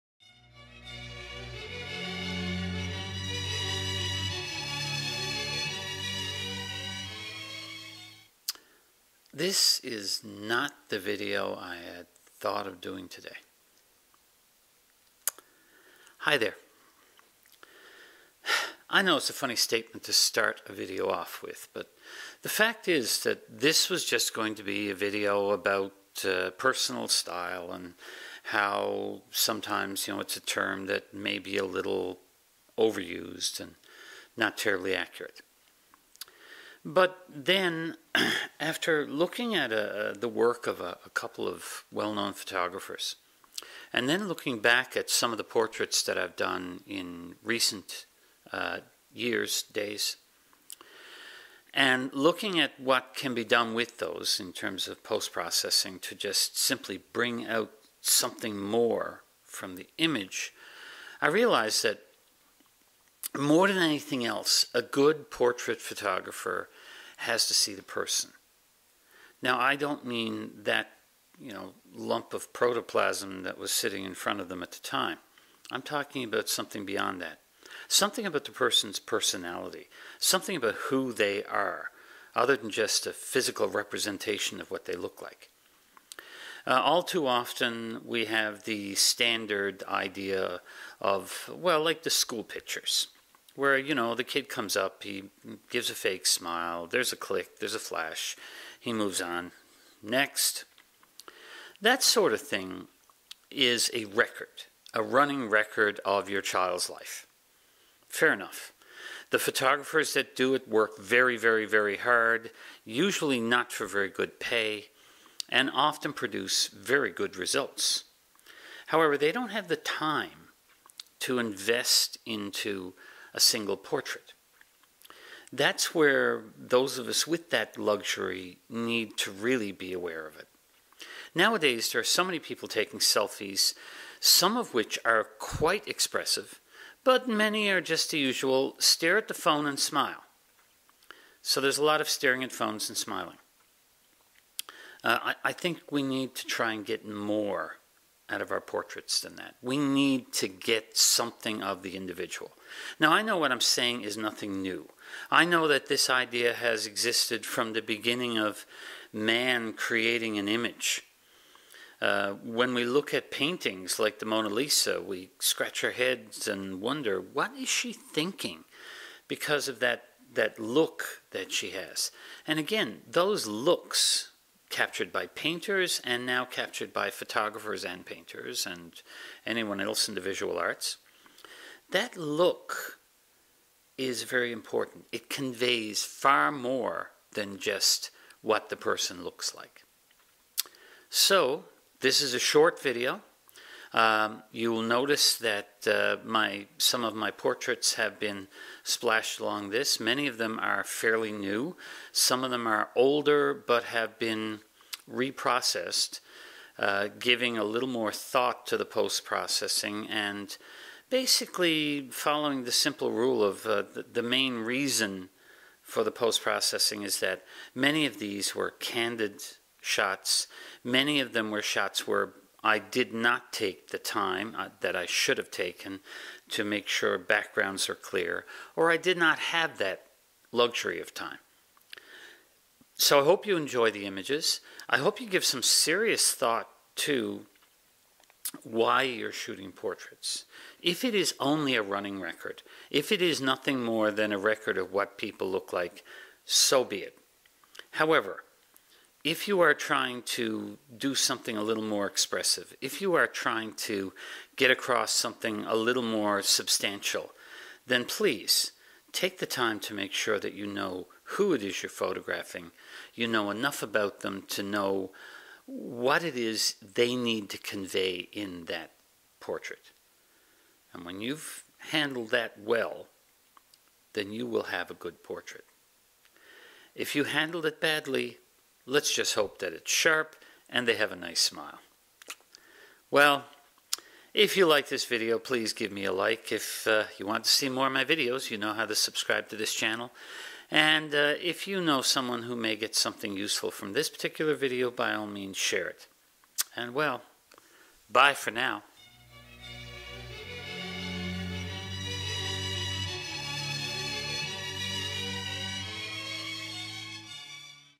Photography 114: Expressive portraits, capturing the personality.
The portrait examples are not high quality image files, sorry.  The point however is to remind everyone that portraits need to be more than just pretty pictures.  Try to capture some aspect of your subject's character for a much more expressive image.